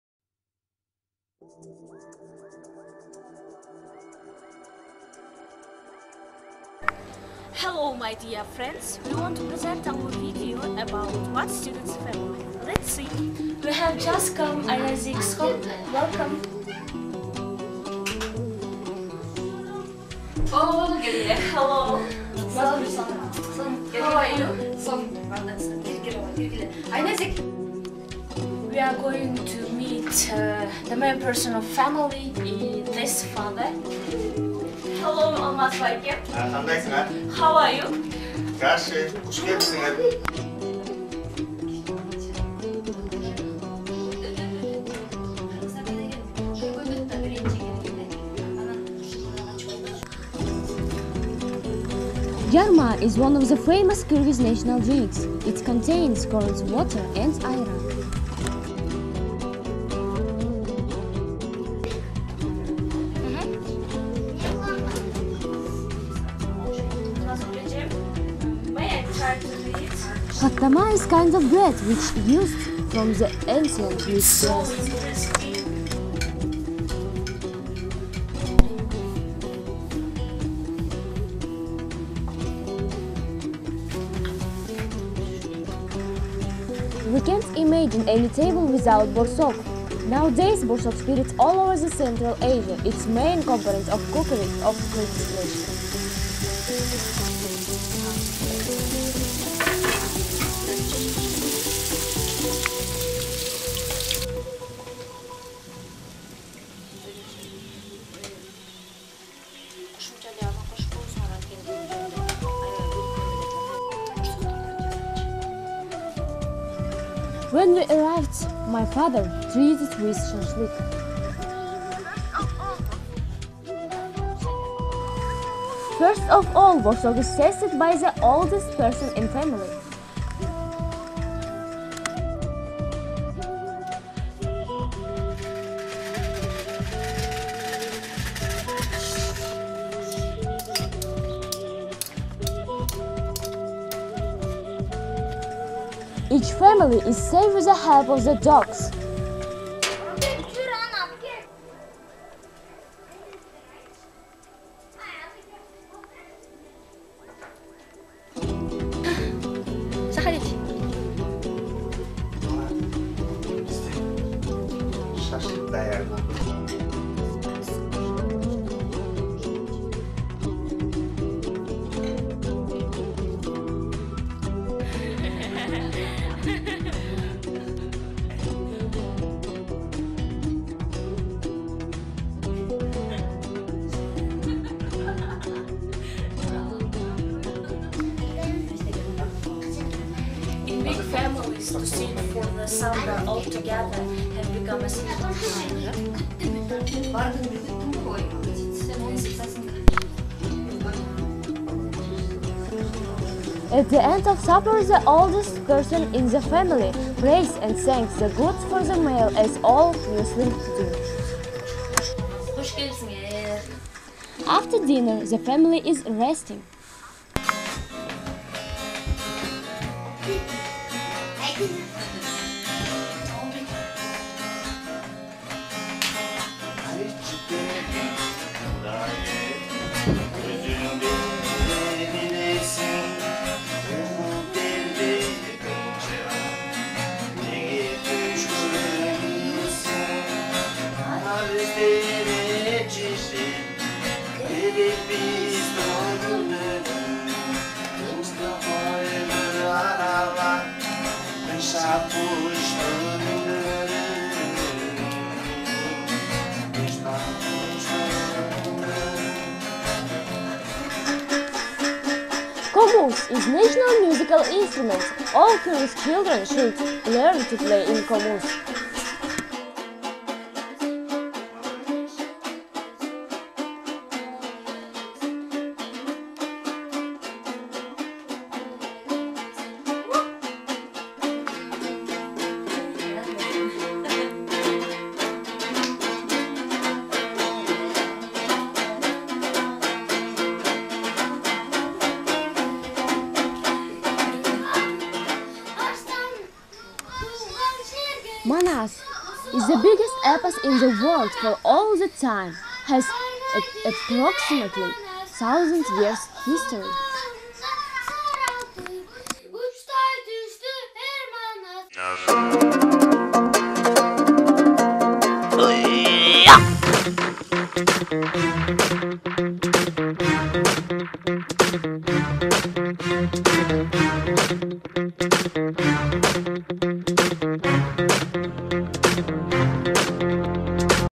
A1-D A Kyrgyz Student's Family
After reading Galsan Tschinag's emotive novel, The Blue Sky, about a young boy growing up within a nomadic family during the 1940's in Mongolia, students in Bishkek, Kyrgyzstan were asked to connect back to their own cultural roots and express their world through video. This video shows a typical Kyrgyz family enjoying a traditional Kyrgyz meal.